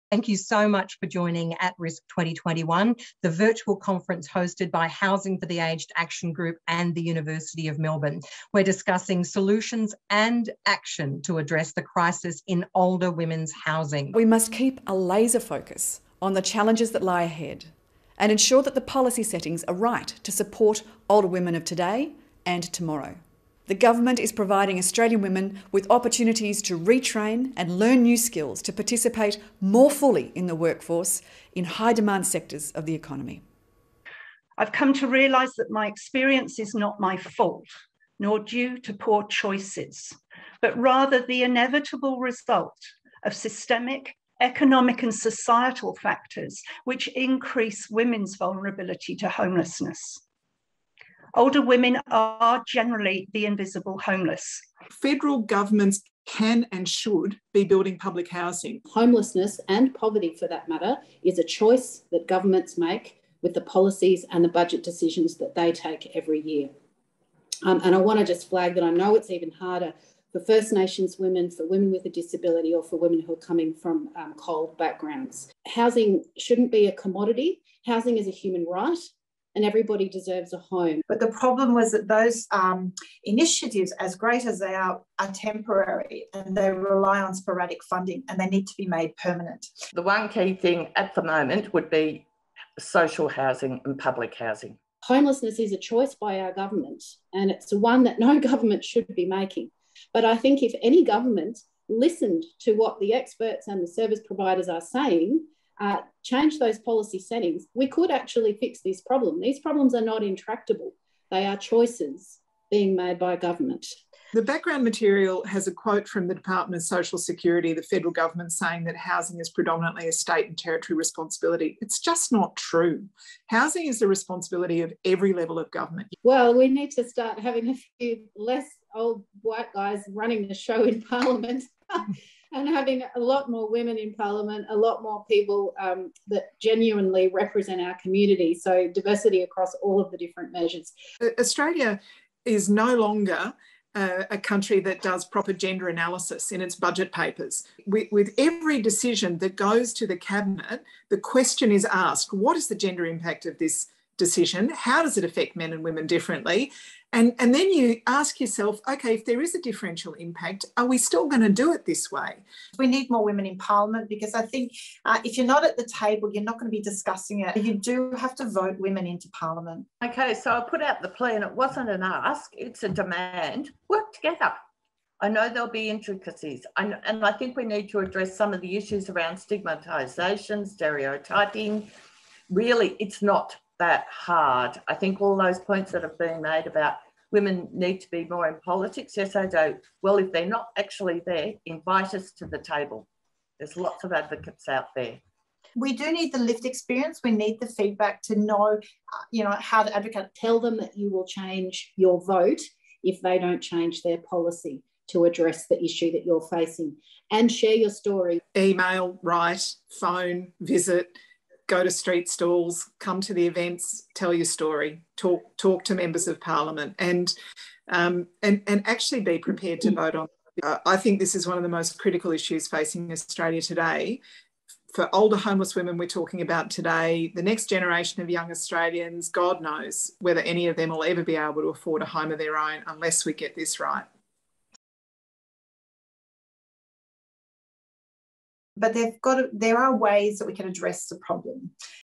At Risk: Older Women Facing Homelessness - Political Panel Highlights
These are short highlights from the At Risk Forum on the 17th of September, featuring lived experience advocates Linda Hahn and Penny Leimhuis Senator Jane Hume, Tanya Plibersek MP, Senator Larissa Waters and Zali Steggall MP.  The full video from the event is available